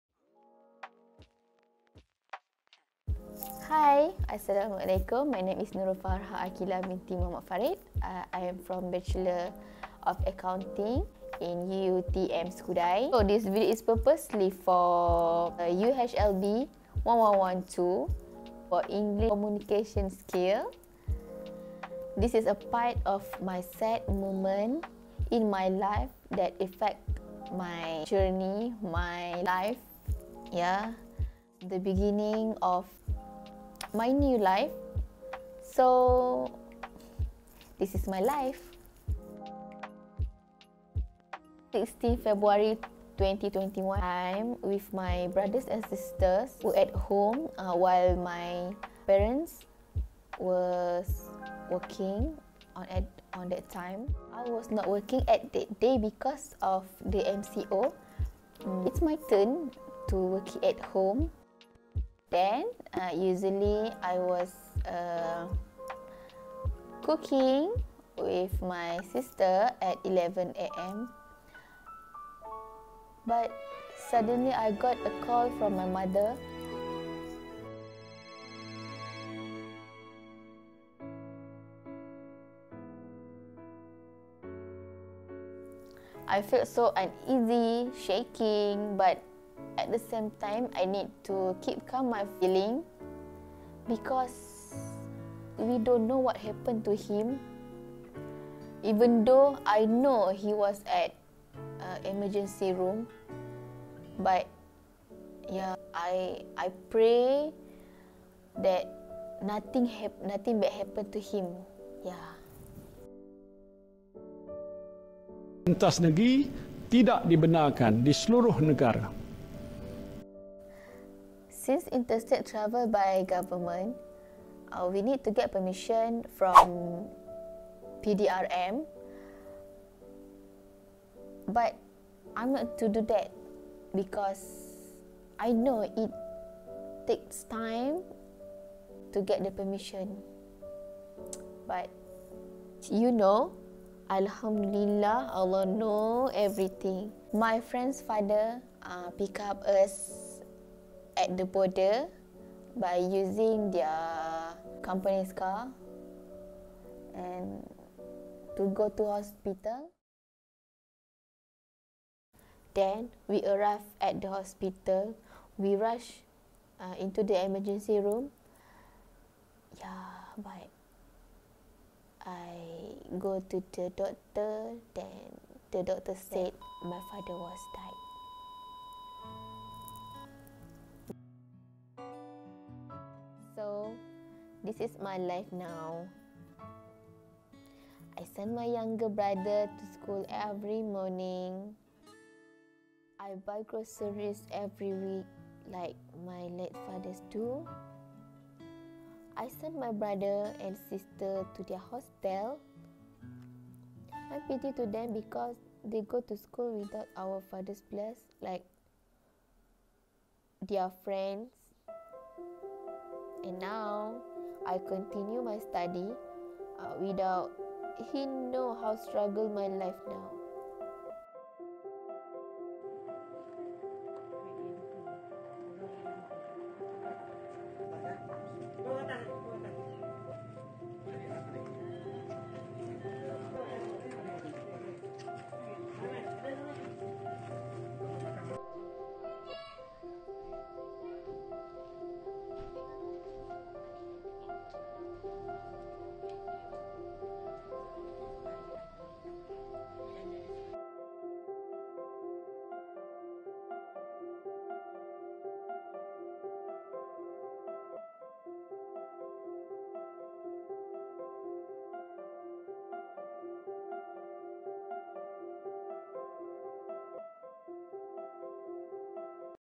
Experiences and Activities during the Movement Control Order 2020 - 2021
Subject: UHLB1112 English Communication Skills (Section 13)
Lecturer: Miss Siti Khadijah Abdul Wahab
Name: Nurul Farhah Aqilah Binti Mohd Farid
Matric No: SX201772BSCHF01

WHAT IS THIS VLOG ABOUT ?
This vlog is for my enrolled in the UHLB1112, English Communication Skills class under my cutes lecturer, Cik Siti Khadijah Abdul Wahab.

AIM OF CREATING THIS VIDEO
In this class, I am required to create content in the form of a web-based project, specifically a vlog on my experiences and activities during the Movement Control Order. Hence, I took the initiative to experience creating content so as to understand the challenges that my experience in coming up with one.

MY CHALLENGES
My first challenge is to find what content and which part that I want to share. I choose to share about this experience because it is the moment that hard to forget during the MCO. I want to express everything so that I can move on with my life. But Alhamdulillah, thanks to my bestfriend Nur Shahidah Saiful who was support and help me on making this video.

Then, the other challenge is which application that I want to used. I started editing video with InShot. My references is my bestfriend Shahidah. She taught me how to edit. She also help me to record my video. Later I found that InShot is easy to use. Therefore, I took my unfinish video and transfer it into Adobe Premiere Pro. I used Adobe for edit my voice. So that i able to come up with a clean voice and transition.

CONCLUSION
Being a newcomer in video editing, I find that this project is interesting and it not only helps me to express my experiences, but also add more English content in social media from Malaysia.